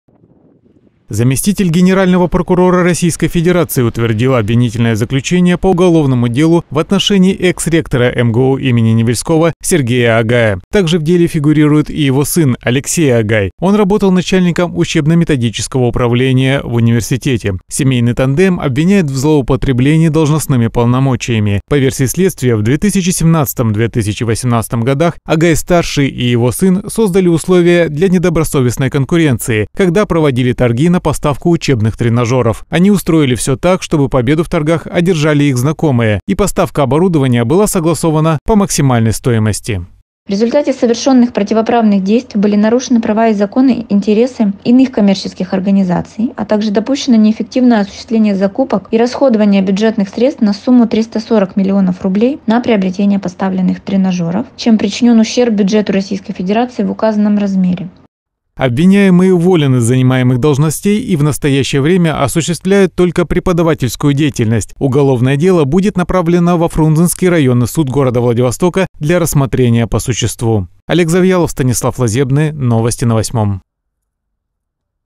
Семейное дело
Закончено расследование в отношении экс-ректора МГУ и утверждено обвинительное заключение.